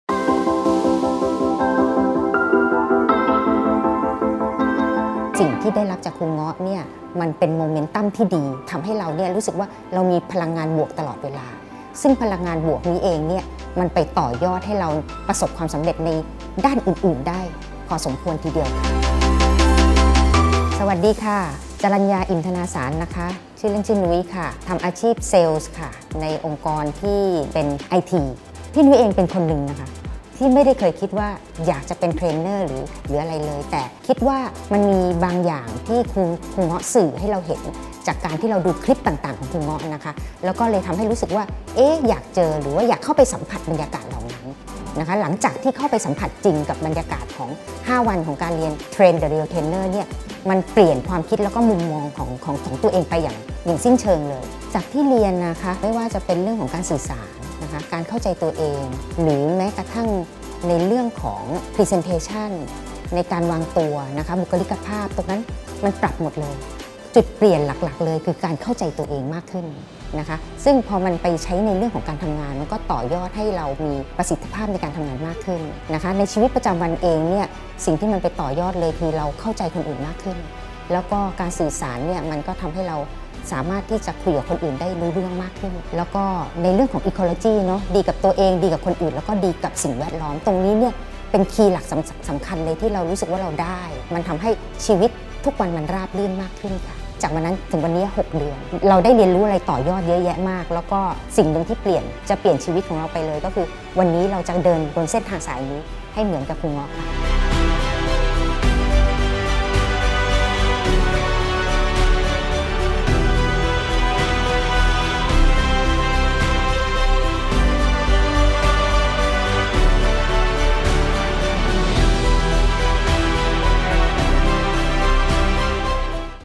Train The Real Trainer - คุณนุ้ย [Voxpop]
คิดบวก แล้วจะประสบความสำเร็จ ?
คิดบวก แล้วชีวิตจะดีขึ้น มันก็เป็นแค่คำโลกสวย ?

เราล้วนใช้ชีวิต
ตามความคิดและความเชื่อของตัวเรา
ยากเป็นไปไม่ได้ จึงไม่ลงมือทำ กับ
ยากแต่เป็นไปได้ ขอลองทำดูก่อน

คิดแค่นี้
แต่ผลลัพธ์ ต่างกันมากมาย

“ การที่เราเข้าใจตัวเอง
เข้าใจที่จะสื่อสารกับคนรอบตัว
รวมถึงบุคลิกภาพ และการวางตัว ที่ดี
มันทำให้เราสามารถต่อยอดไปสู่ความสำเร็จในชีวิตได้จริง
โดยคลาสนี้ มันตอบโจทย์สิ่งนั้นให้กับเรา ”